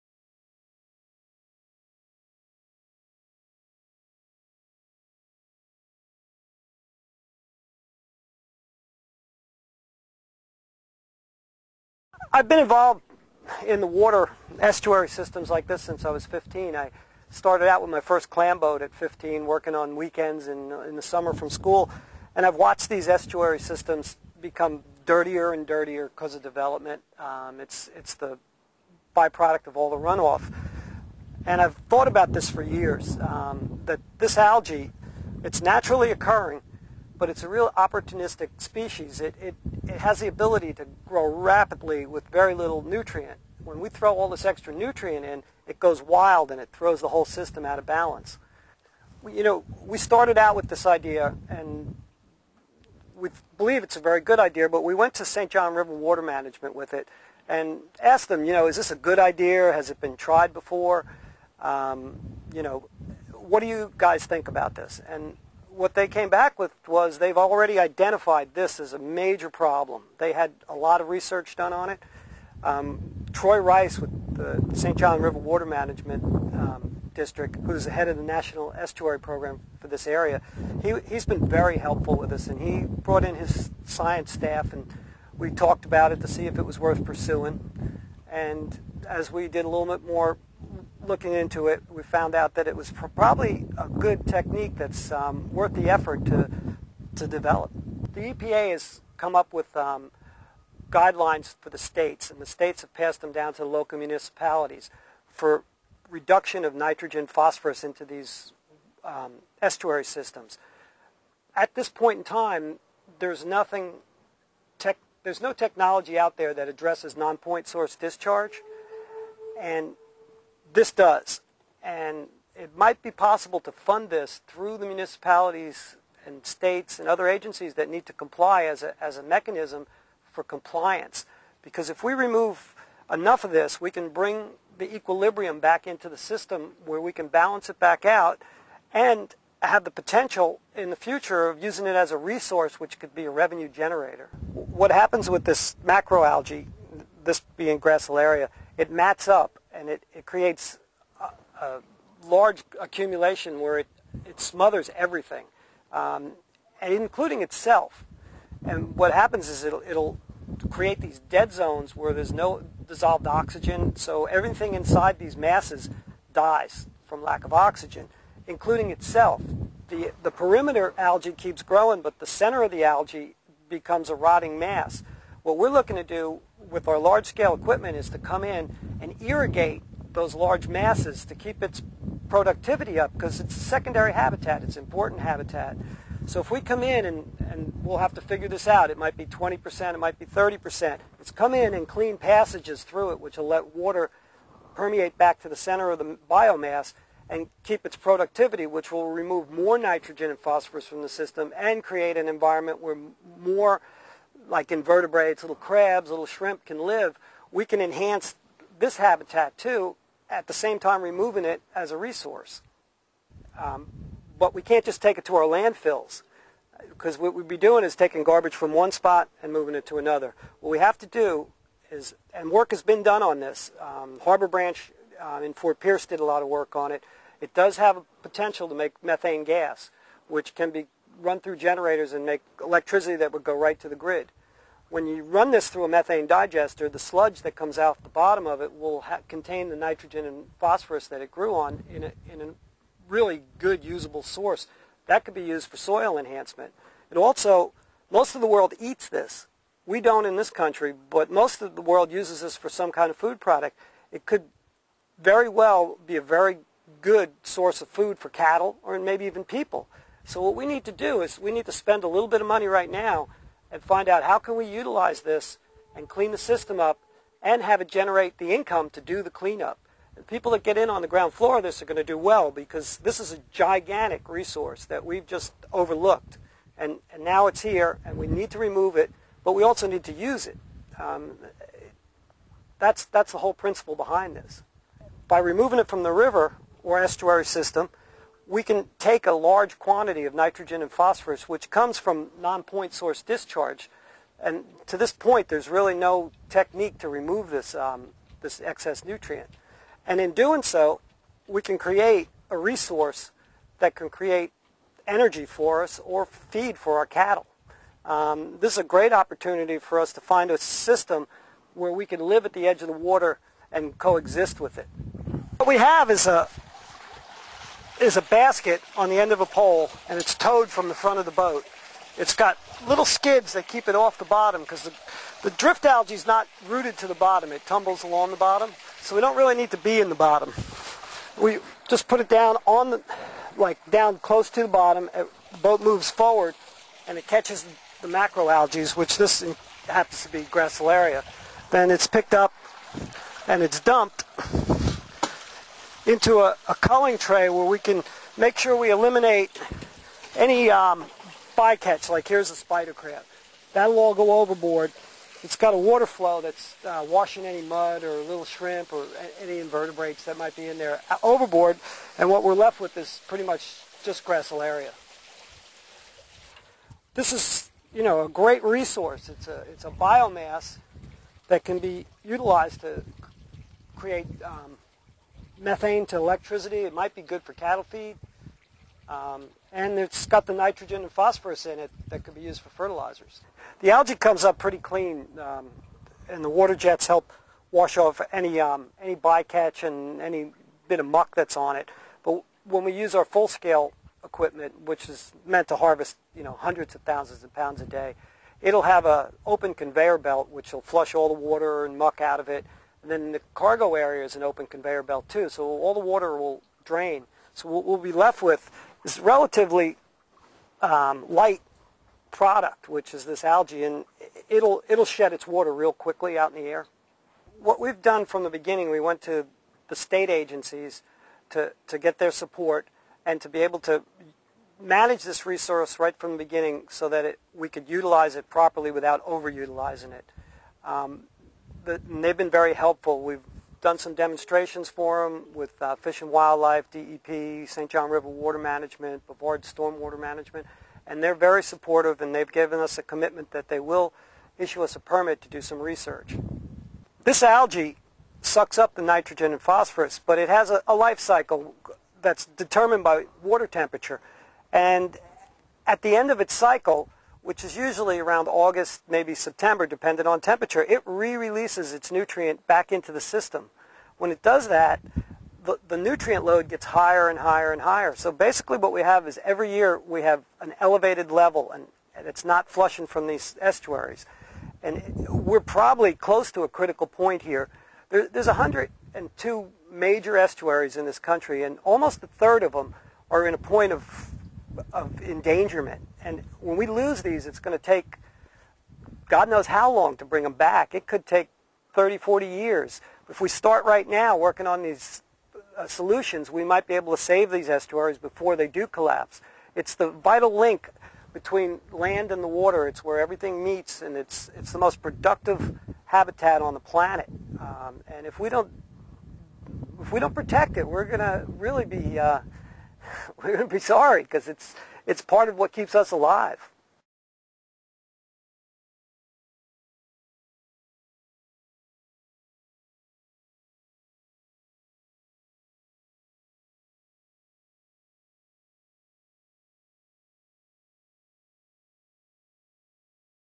Algae Collection Technology :: Harvested Gracilaria, From Nuisance to a Renewable Resource
Harvested Gracilaria
From Nuisance to a Renewable Resource

America's estuaries are an "untapped resource" rich with Gracilaria algae. As development has expanded, massive amounts of non-point source nutrient runoff have been leaking into our estuaries causing excessive blooms of algae that have begun to take over the environment and kill off native species that have been there for centuries. We have found a way to safely harvest sufficient quantities of this Gracilaria to clean up the estuaries, while at the same time creating commercial uses for the algae that will prove profitable in products such as cattle feed, methane, agar, bio-fuel and much more.